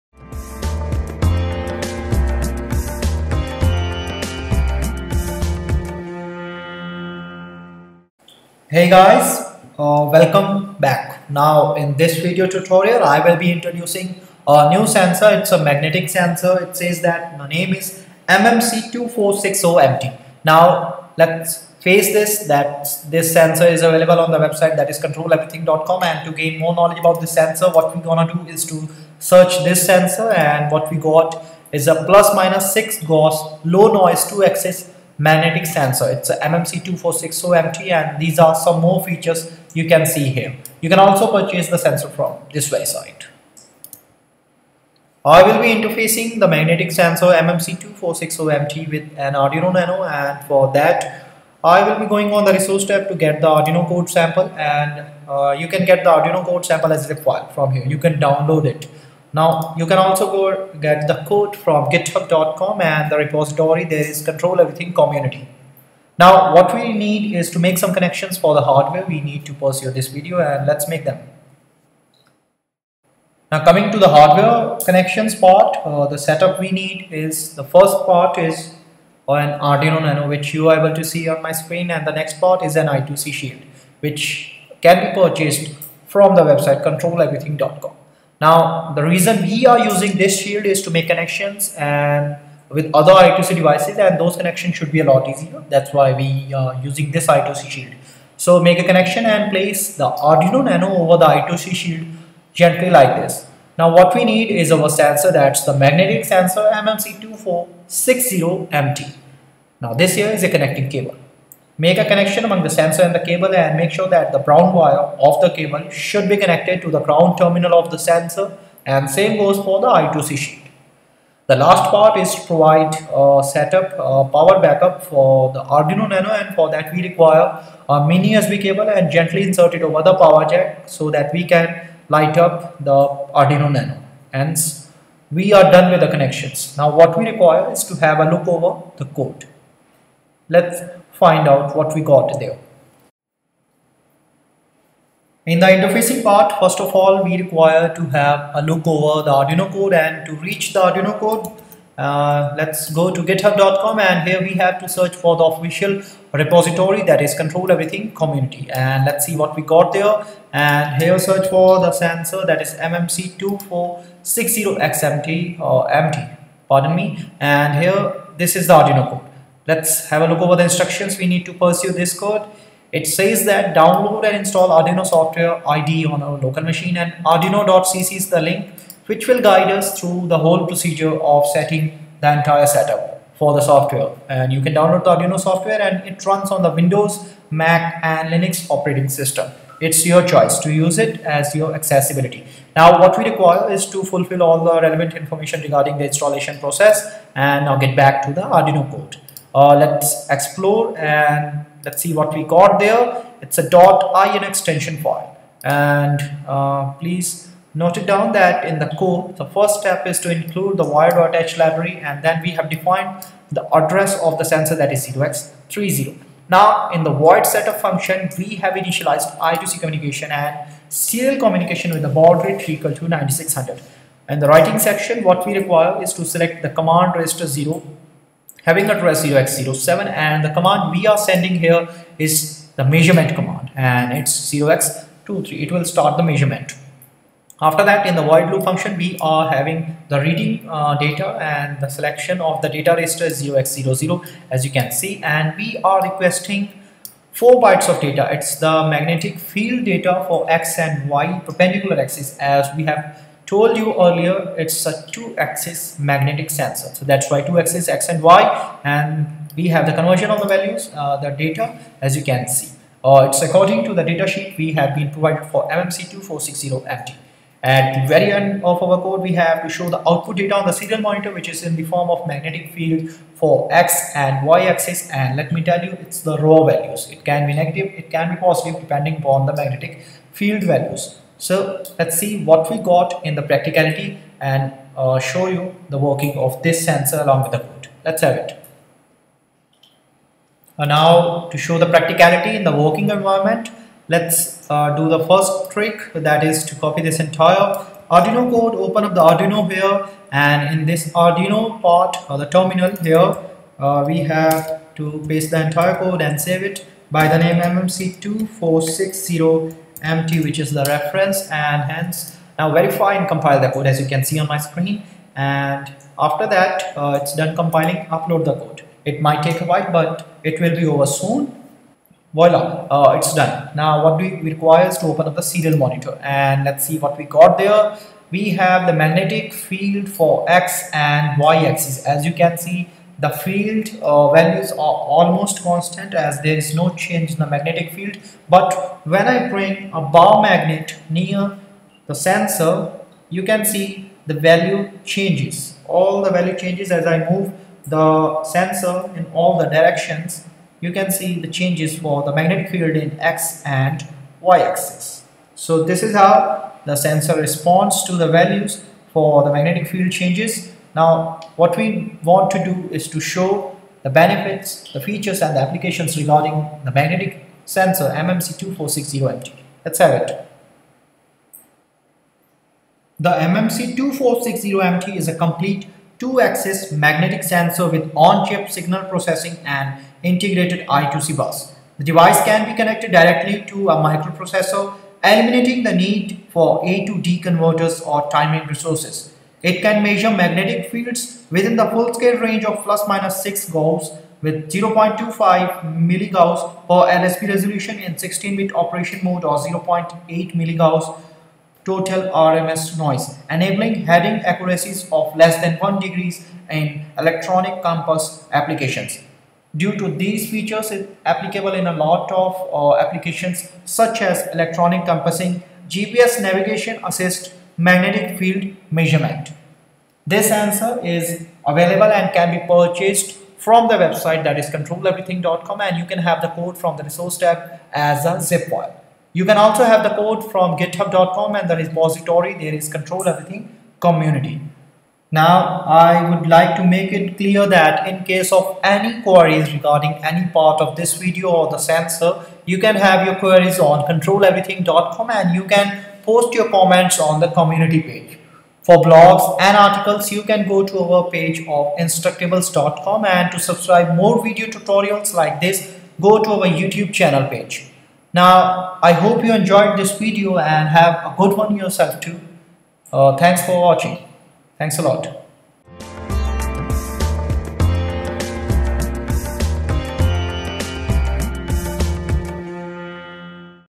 Arduino Nano MMC2460MT 2-Axis Magnetic Sensor Tutorial
MMC2460MT ±6 Gauss Low Noise 2-Axis Magnetic Sensor I²C Mini Module. The MMC246xMT is a complete 2-axis magnetic sensor with on-chip signal processing and integrated I2C bus. The device can be connected directly to a microprocessor, eliminating the need for A/D converters or timing resources. It can measure magnetic fields within the full scale range of ±6 G, with 0.25 mG per LSB resolution in 16 bit operation mode and 0.8 mG total RMS noise, enabling heading accuracies of less than 1 degree in electronic compass applications.
Applications :- Electronic Compassing, GPS Navigation Assist, Magnetic Field Measurement.

Related Links :-